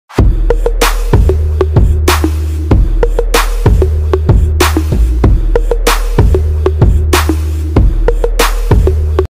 Custom sports slippers | Men style sandals with logo | Wholesale
You can sent your logo to us so that we can make an effect picture for you to confirm. We can provide print, embossed, dripping( Drop plastic) and other processes.